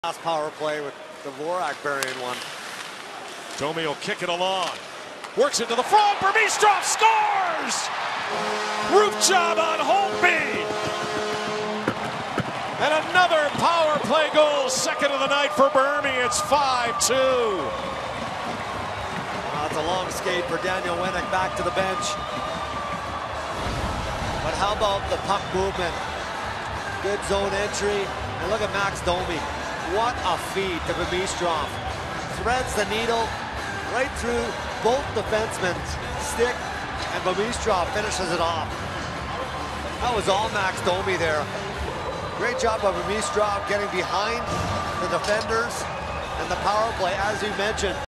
Alex Burmistrov's nifty goal vs Capitals (31 mar 2017)
Гол Александра Бурмистрова в ворота "Кэпиталс" за "Аризону".